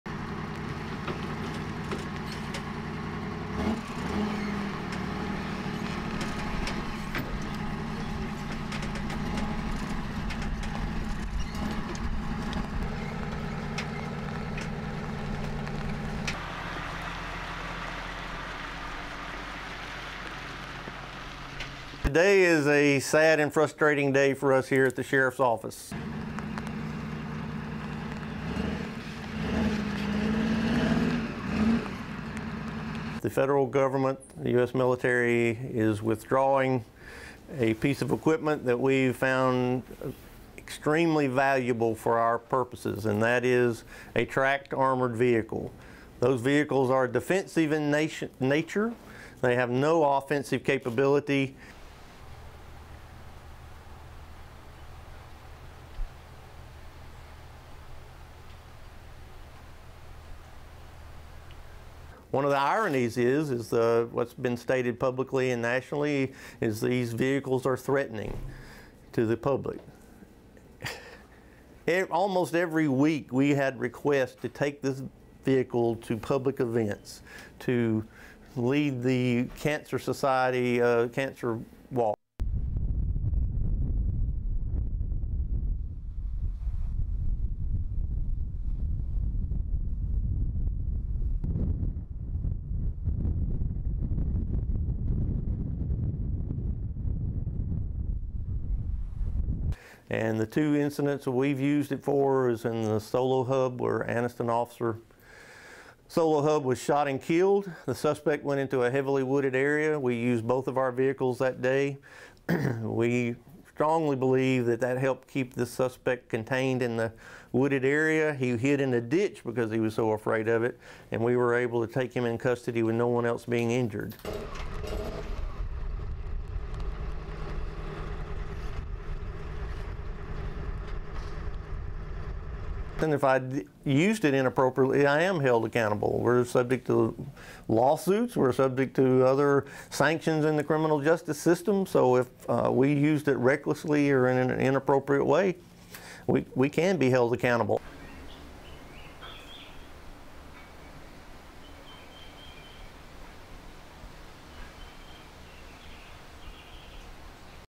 Sheriff Upset Over Armored Tank Take Back
(18 Nov 2015) The federal government has reclaimed dozens of tank-like tracked vehicles, grenade launchers and even bayonets from local police agencies since President Barack Obama issued an order to demilitarize civilian law enforcement in January.
The White House recall has now come to one Southern county, and the sheriff isn't happy.
Calhoun County Sheriff Larry Amerson says his county was the first in Alabama to obtain a free armored personnel carrier from the Pentagon under a surplus program in 2001. And his county is now the first in the state to have the military show up to reclaim one of those vehicles under Obama's orders.
A military contractor loaded up and hauled away one of the sheriff department's two tracked personnel carriers on Wednesday.
Amerson says his department has used the vehicles twice in 14 years to capture dangerous suspects. That includes a man who hid in a ditch after a local police officer, Justin Sollohub, was shot to death in 2011.
Critics say it's intimidating for local police to have military equipment. But Amerson says he gets requests constantly from community members wanting one of the vehicles to participate in a parade or other event.
Obama's order came after contentious protests in Ferguson, Missouri.
Amerson says his department can't afford to buy a carrier like the one seized by the federal government, and he fears officers won't be as safe in the future.